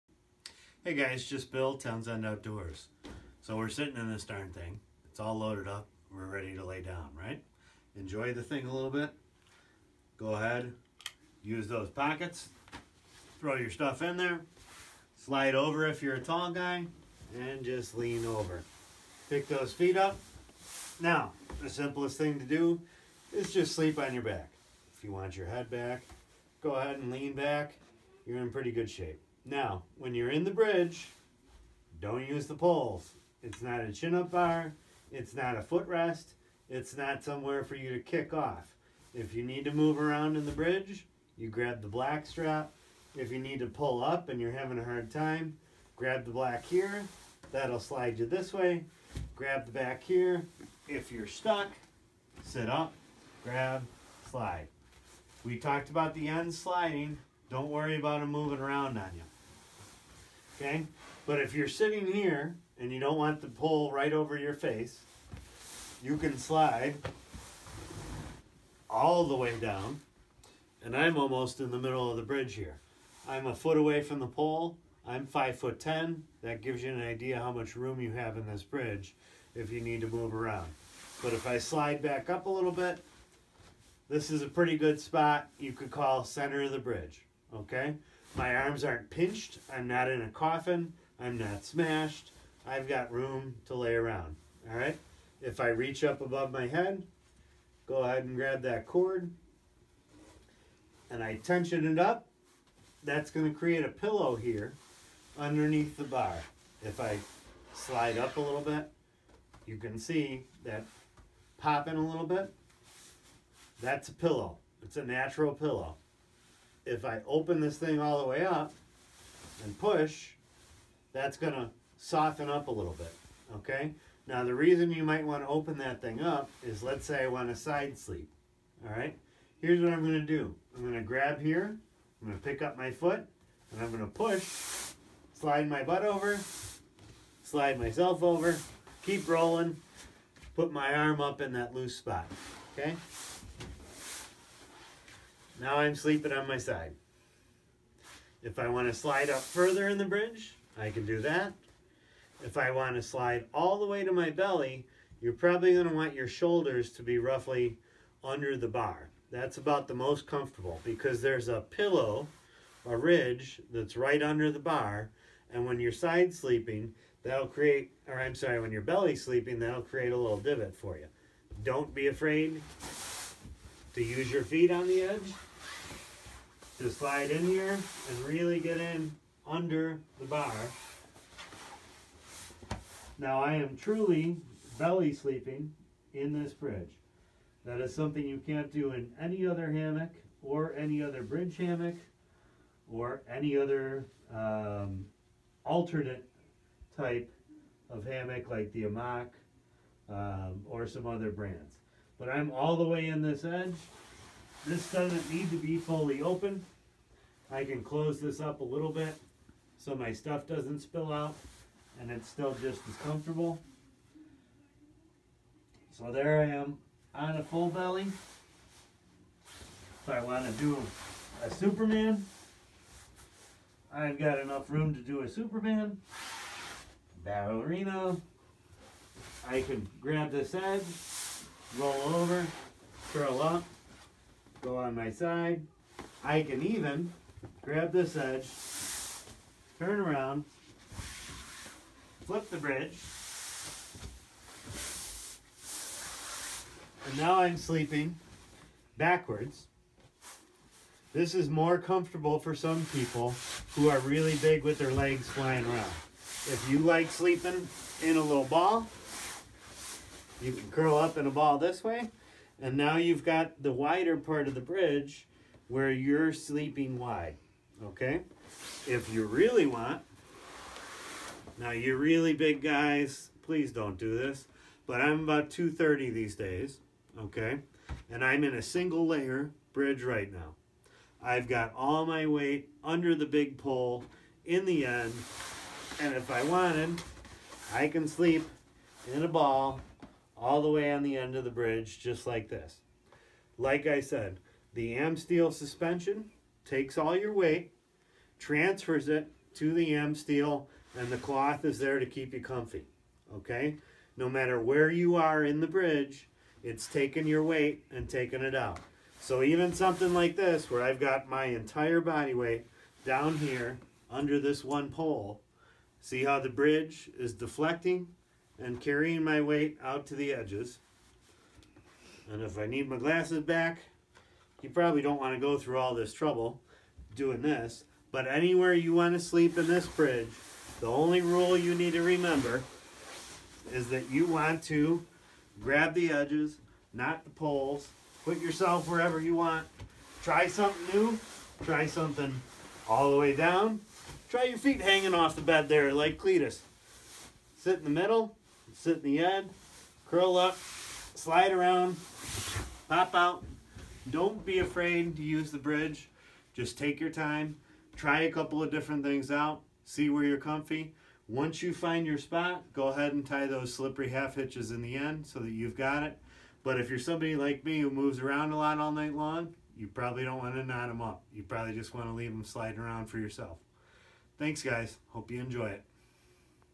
BGLux Setup 9 Using the darn thing
Finally!  Let's see how to laydown and get comfy.